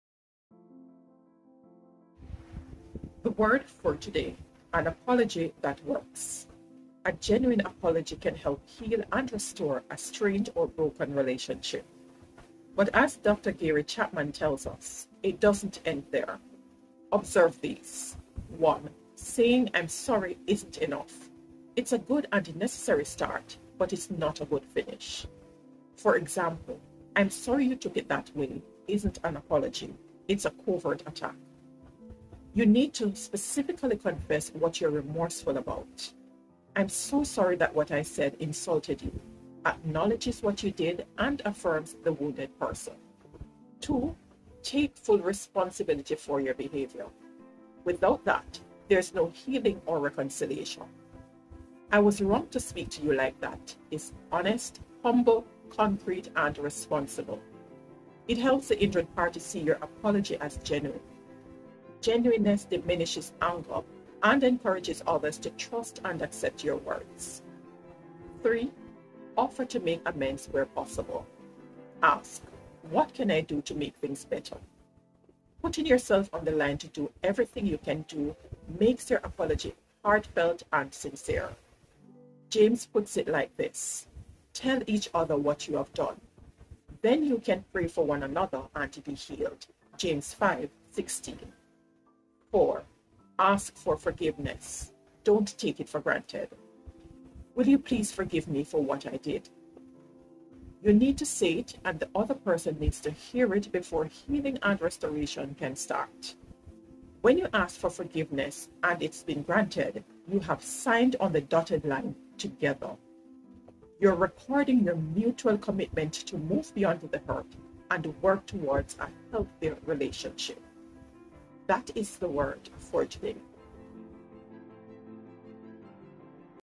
Devotional | AN APOLOGY THAT WORKS
Presented by Winsome Sherrier Witter @SERENITYResourceConnector

A genuine apology can help heal and restore a strained or broken relationship. But as Dr Gary Chapman tells us, it doesn't end there.

Observe these: (1) Saying 'I'm sorry' isn't enough! It's a good and necessary start, but it's not a good finish. For example, 'I'm sorry you took it that way' isn't an apology; it's a covert attack! You need to specifically confess what you're remorseful about. 'I'm so sorry that what I said insulted you' acknowledges what you did and affirms the wounded person. (2) Take full responsibility for your behaviour. Without that, there is no healing or reconciliation. 'I was wrong to speak to you like that' is honest, humble, concrete and responsible. It helps the injured party see your apology as genuine. Genuineness diminishes anger and encourages others to trust and accept your words.

(3) Offer to make amends where possible. Ask, 'What can I do to make things better?' Putting yourself on the line to do everything you can do makes your apology heartfelt and sincere. James puts it like this: 'Tell each other what you have done. Then you can pray for one another and be healed' (James 5:16 NCV). (4) Ask for forgiveness - don't take it for granted. 'Will you please forgive me for what I did?' You need to say it and the other person needs to hear it before healing and restoration can start. When you ask for forgiveness and it's been granted, you have 'signed on the dotted line' together. You're recording your mutual commitment to move beyond the hurt and work towards a healthier relationship.

Music from vivavideo